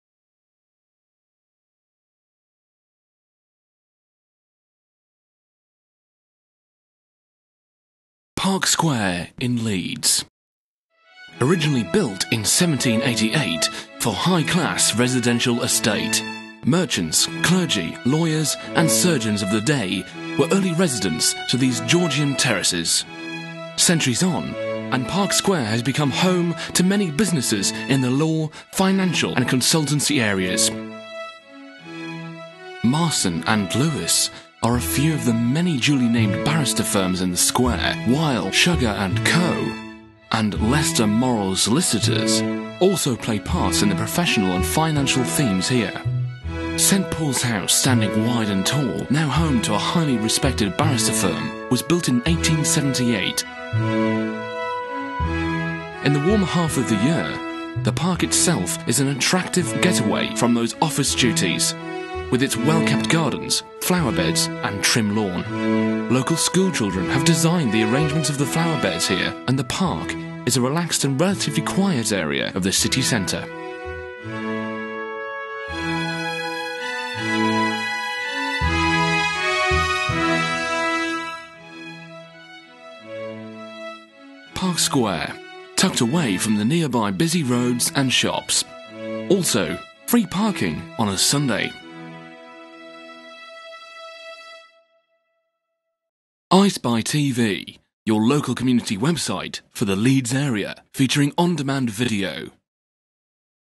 iSPI TV - Park Square in Leeds
Park Square in Leeds is a well preserved area in the centre of Leeds town. Take a look at what is tucked away just of the Headrow in Leeds.